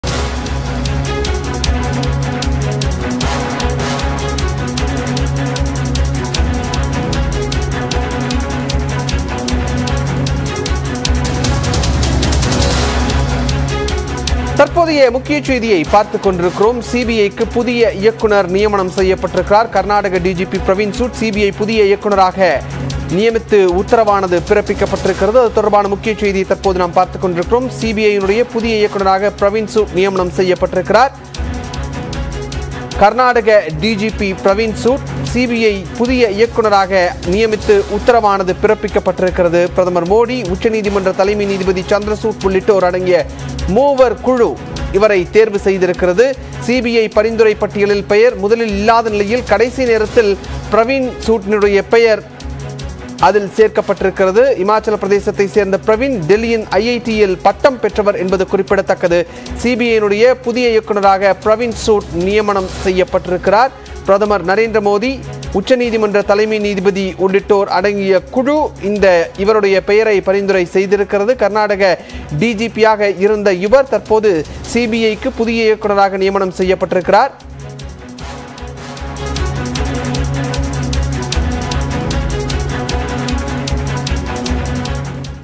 Breaking News | சிபிஐ புதிய இயக்குனராக பிரவீன் சூட் நியமனம் | CBI | Praveen Sood | Tamil News
சிபிஐ புதிய இயக்குனராக பிரவீன் சூட் நியமனம் | செய்திகளை துல்லியமாகவும் துரிதமாகவும் வழங்கும் தமிழகத்தின் முதன்மை செய்தி தொலைக்காட்சி உங்கள் நியூஸ்18 தமிழ்நாடு.

News18TamilNadu | CBI | Karnataka DGP Praveen Sood Tamil News